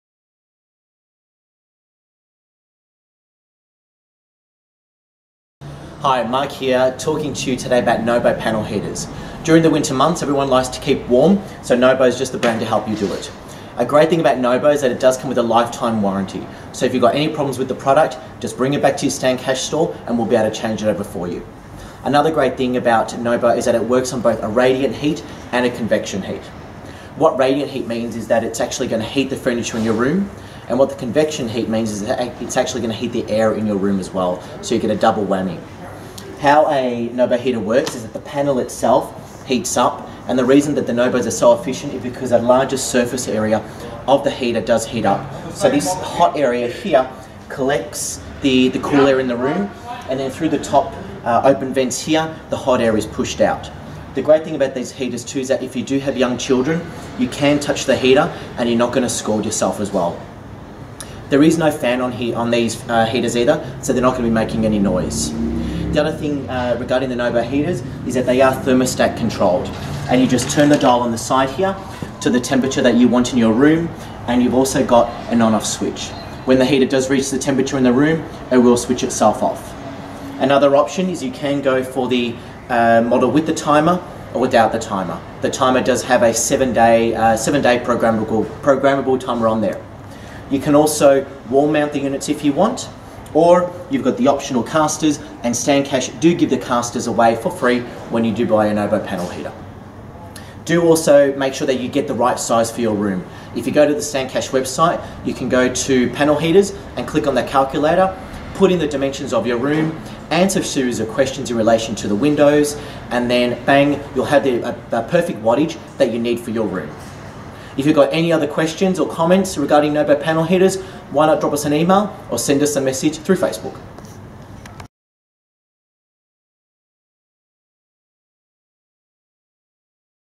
Stan Cash presents the NOBO heater
Slim & stylish design

• Manufactured in Norway since 1947
• Safe
• Completely silent
• Accurate temperature control with electronic thermostat
• Economical
• Clean
• Efficient
• Healthy
• Unobtrusive
• Ideal for sufferers of asthma, hay fever, and allergies.
• Combined radiant and convection heat provides gentle and even warmth.
• Optional legs with castors available for all models
• Full 7 YEAR manufacturer’s warranty plus an outstanding 10 YEAR manufacturer’s warranty on the heating element.